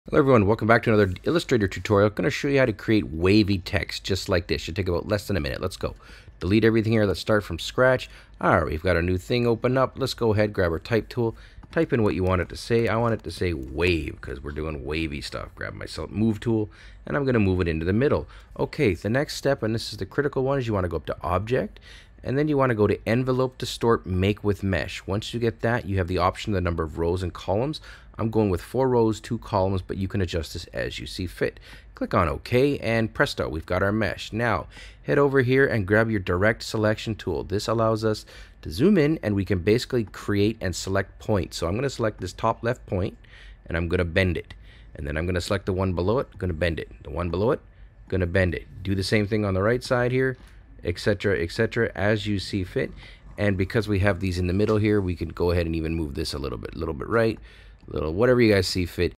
Create Wavy Text in Adobe Illustrator - Short Tutorial
In this quick tutorial I show you how to create wavy, or bendy, text in Adobe Illustrator 2023.  Step one is to create a new project and add your text.  Next, go to Object -- Envelope Distort -- Make With Mesh.  Select four columns and two rows.  Next, grab your Direct Selection Tool and move the points on the edges of the mesh to make your text wavy!